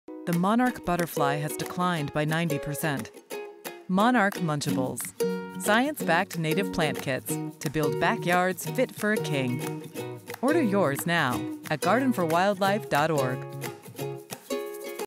Build a Backyard Fit for a King
The monarch butterfly has declined by 90%. Our science-backed native plant kit, Monarch Munchables, give you the power to help reverse that trend by creating habitat for monarch butterflies and other important pollinators and wildlife in your own backyard (or patio, porch, rooftop, etc.). Get a curated collection of native plants for full-sun or part-shade conditions, along with all the tips you need for wildlife gardening success, at shop.gardenforwildlife.org. All sales support the National Wildlife Federation's non-profit mission to help wildlife thrive in an ever-changing world.